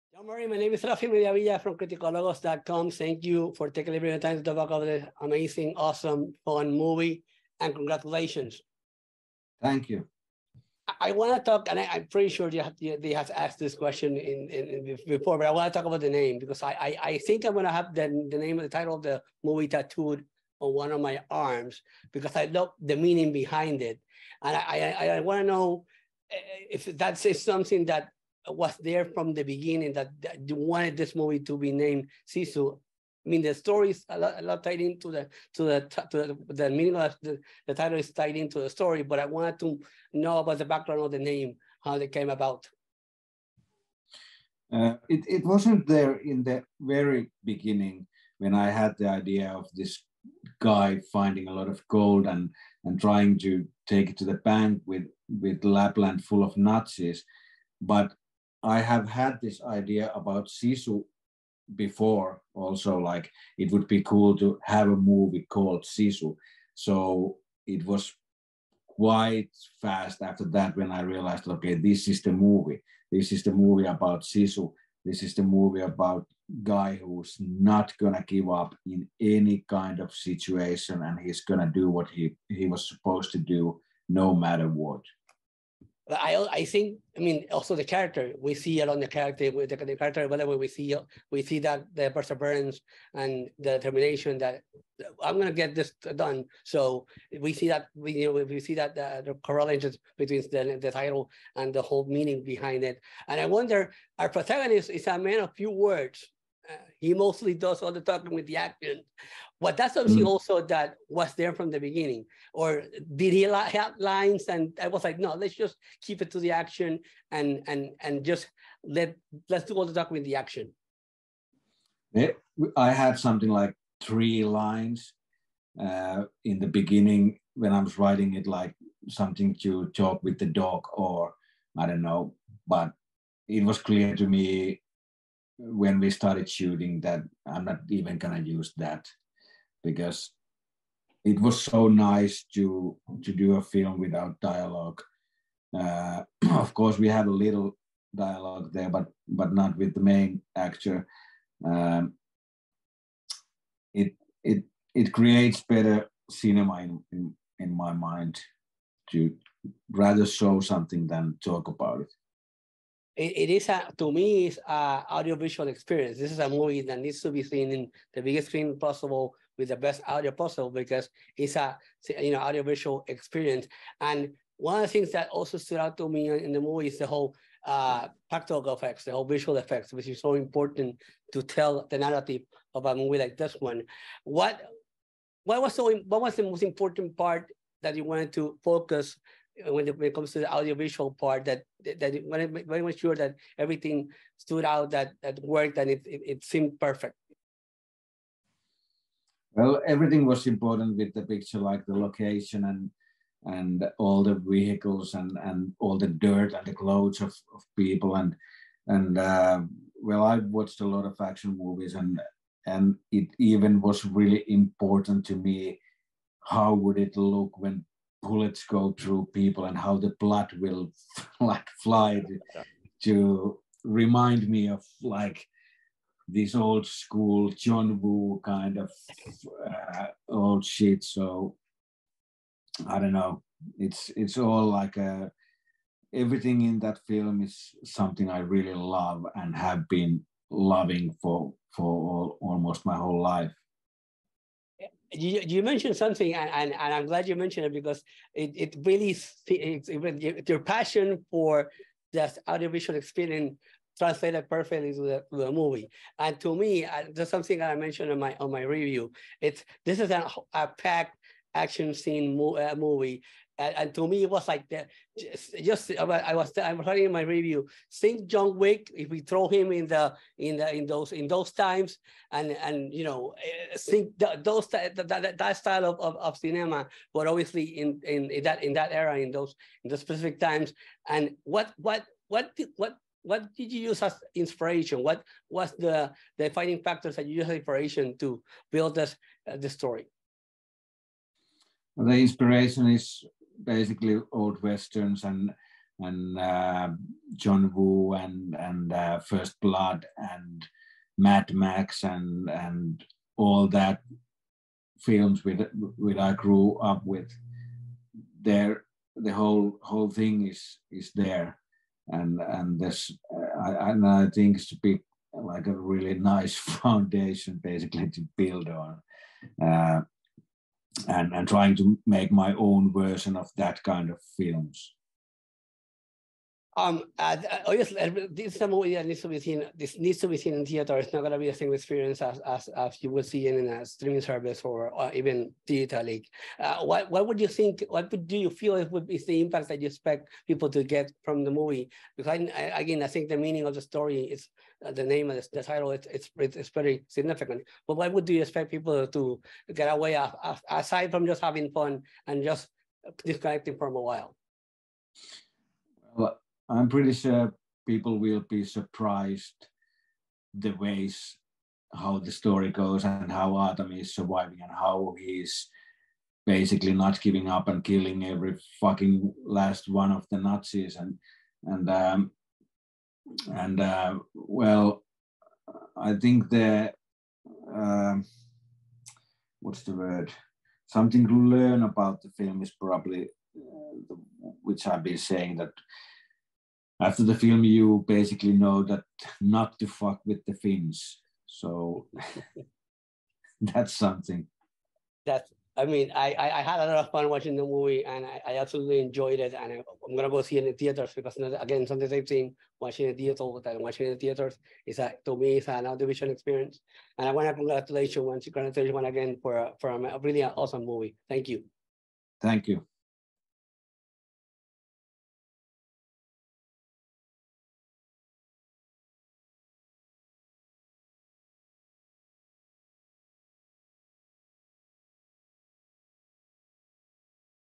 A Nazi Killing John Wick - Interview "SISU" writer/director Jalmari Helander
A Nazi Killing John Wick - Interview "SISU" writer/director Jalmari Helander. Out April 28 In Selected Theaters.

During the last desperate days of WWII, a solitary prospector (Jorma Tommila) crosses paths with Nazis on a scorched-earth retreat in northern Finland. When the Nazis steal his gold, they quickly discover that they have just tangled with no ordinary miner. While there is no direct translation for the Finnish word “sisu”, this legendary ex-commando will embody what sisu means: a white-knuckled form of courage and unimaginable determination in the face of overwhelming odds. And no matter what the Nazis throw at him, the one-man death squad will go to outrageous lengths to get his gold back – even if it means killing every last Nazi in his path.